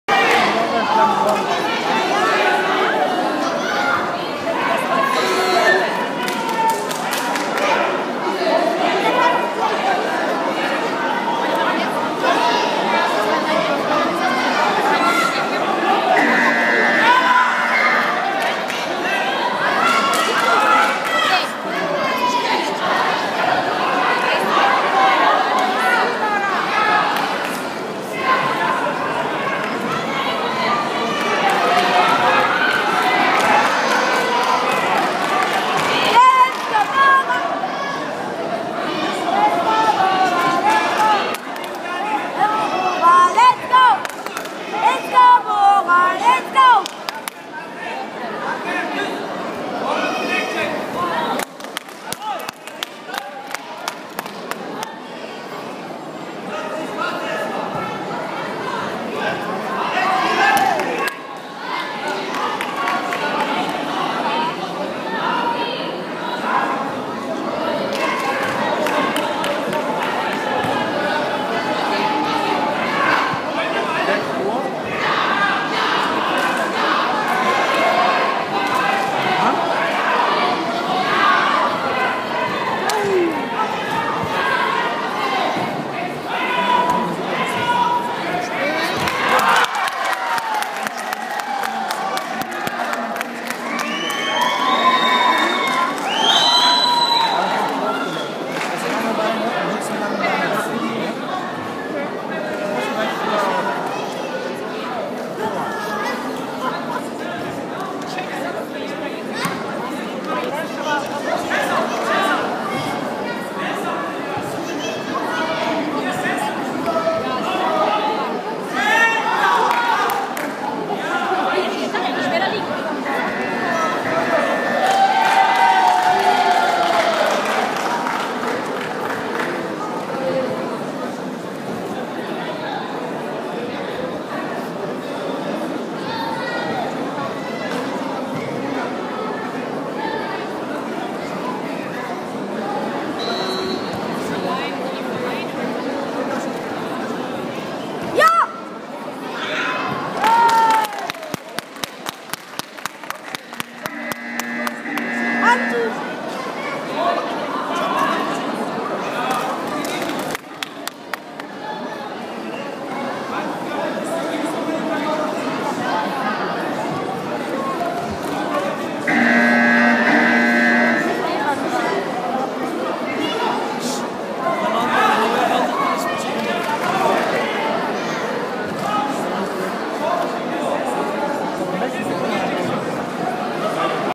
Bergisch Open 2013 Bora Cinar 1. Kampf 2. Runde KO
Bora Cinar blaue Weste mach einen KO